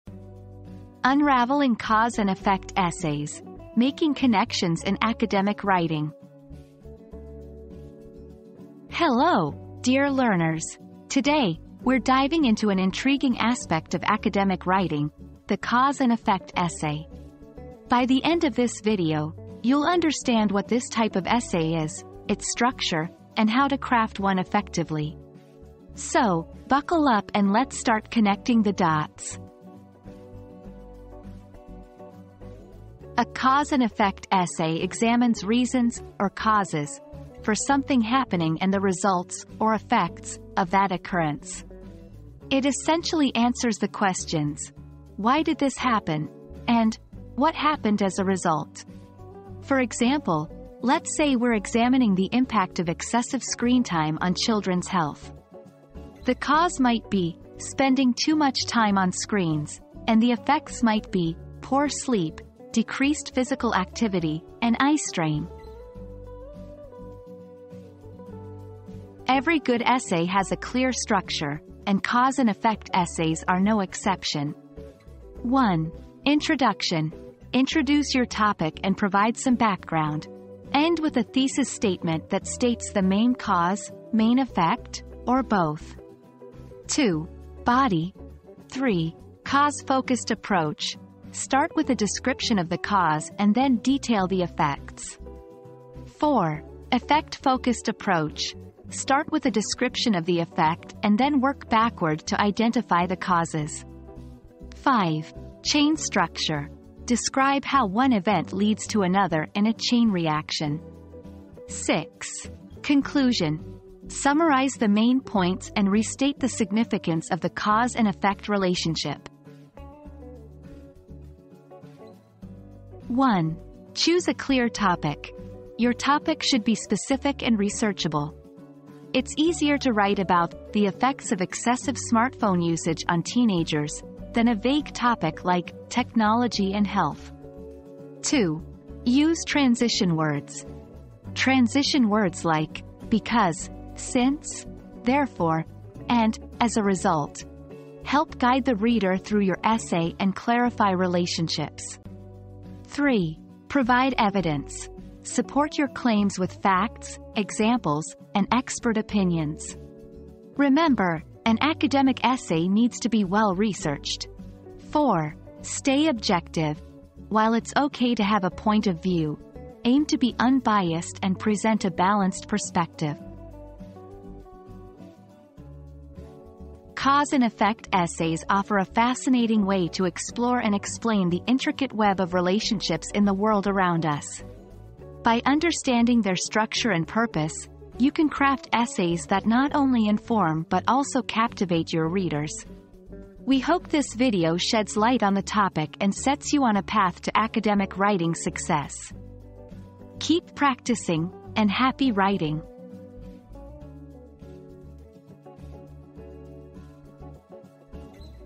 Unraveling Cause and Effect Essays: Making Connections in Academic Writing
Unraveling Cause and Effect Essays: Making Connections in Academic Writing • Learn all about cause and effect essays in this informative video! Discover what they are, their structure, and how to craft a stellar one. Dive into the world of academic writing and start connecting the dots!

00:00 • Introduction - Unraveling Cause and Effect Essays: Making Connections in Academic Writing
00:32 • What is a Cause and Effect Essay?
01:08 • Structure of a Cause and Effect Essay
02:02 • Tips for Crafting a Stellar Cause and Effect Essay